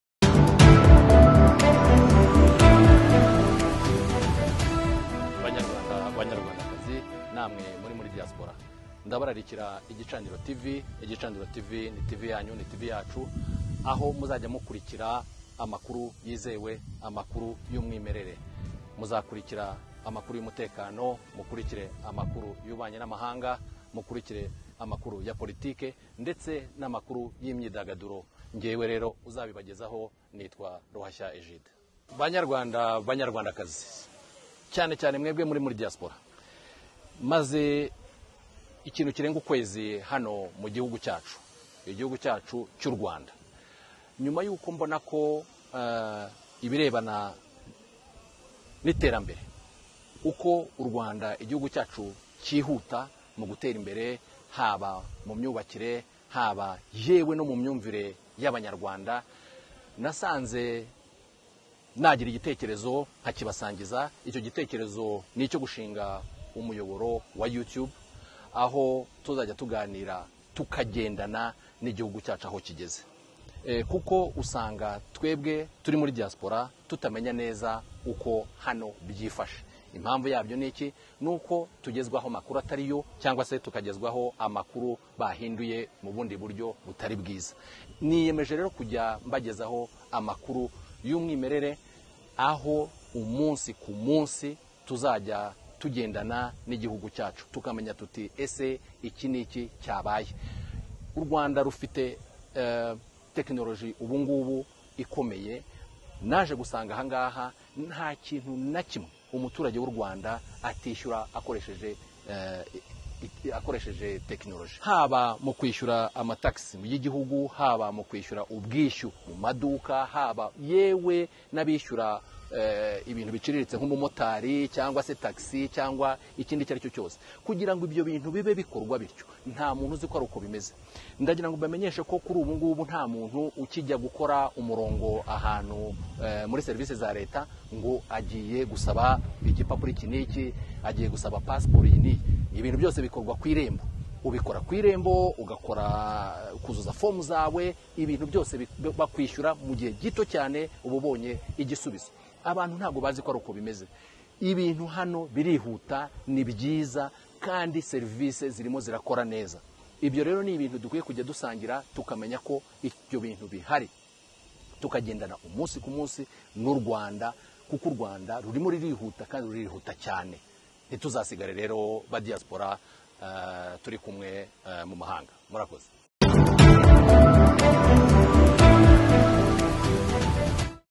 IGICANIRO TV Introduction - Izingiro ry'Amakuru, Isesengura no Gukunda u Rwanda
IGICANIRO TV - Izingiro ry'amakuru, isesengura no gukunda u Rwanda - (News Channel, Analysis & Patriotism)
Igicaniro TV is a new TV Channel for Rwandans in Rwanda and Rwandans in Diaspora especially. In this channel you will follow news on security in Rwanda, Rwanda foreign affairs with other countries, Rwandan Politics and news on Showbiz and entertainment.
Rwanda is fast developing in every aspect such as the use of technology, in construction industry, health to name few. The mobile use in Rwanda is fascinating at the moment, most people use their mobile phones to pay their taxes, pay goods, transfer money among themselves, even be able to apply for certain documents such as passport. There are new modern buildings being elevated in the capital city, new residentials area being build, renovation taking place in other areas… People easily can get access to health facilities across the country.
For Rwandans living in Diaspora sometimes miss on the progress the country is making, sometimes there are misinformation on what’s happening in Rwanda from those who don’t want to see or don’t like the progress Rwanda is doing. This is the reason why Igicaniro TV has been formed to keep you updated with the real progress Rwanda is making.

IN FRENCH
Igicaniro TV est une nouvelle chaîne de télévision pour les Rwandais au Rwanda et les Rwandais de la diaspora en particulier. Dans cette chaîne, vous suivrez les nouvelles sur la sécurité au Rwanda, les affaires étrangères du Rwanda avec d'autres pays, la politique rwandaise et les nouvelles sur le showbiz et le divertissement.
Le Rwanda se développe rapidement dans tous les aspects tels que l'utilisation de la technologie, dans l'industrie de la construction, la santé pour n'en nommer que quelques-uns. L'utilisation du mobile au Rwanda est fascinante en ce moment, la plupart des gens utilisent leurs téléphones portables pour payer leurs impôts, payer des marchandises, transférer de l'argent entre eux, voire pouvoir demander certains documents comme le passeport. Il y a de nouveaux bâtiments modernes en construction dans la capitale, de nouvelles zones résidentielles en construction, des rénovations en cours dans d'autres zones… Les gens peuvent facilement accéder aux établissements de santé à travers le pays.
Pour les Rwandais vivant dans la diaspora, ils manquent parfois les progrès réalisés par le pays, parfois il y a de la désinformation sur ce qui se passe au Rwanda de la part de ceux qui ne veulent pas voir ou n'aiment pas les progrès que le Rwanda fait. C'est la raison pour laquelle Igicaniro TV a été créée pour vous tenir au courant des progrès réels réalisés par le Rwanda.

In Spanish
Igicaniro TV es un nuevo canal de televisión para los ruandeses en Ruanda y especialmente para los ruandeses en la Diáspora. En este canal podrá seguir las noticias sobre seguridad en Ruanda, asuntos exteriores de Ruanda con otros países, política ruandesa y noticias sobre el mundo del espectáculo y el entretenimiento.
Ruanda se está desarrollando rápidamente en todos los aspectos, como el uso de la tecnología, la industria de la construcción, la salud, por nombrar algunos. El uso del móvil en Ruanda es fascinante en este momento, la mayoría de la gente usa sus teléfonos móviles para pagar sus impuestos, pagar bienes, transferir dinero entre ellos, incluso poder solicitar ciertos documentos como el pasaporte. Se están elevando nuevos edificios modernos en la ciudad capital, se están construyendo nuevas áreas residenciales, se están realizando renovaciones en otras áreas ... La gente puede acceder fácilmente a las instalaciones de salud en todo el país.
Para los ruandeses que viven en la diáspora a veces se pierden el progreso que está haciendo el país, a veces hay información errónea sobre lo que está sucediendo en Ruanda por parte de aquellos que no quieren ver o no les gusta el progreso que está haciendo Ruanda. Esta es la razón por la que Igicaniro TV se ha creado para mantenerlo actualizado con el progreso real que está haciendo Ruanda.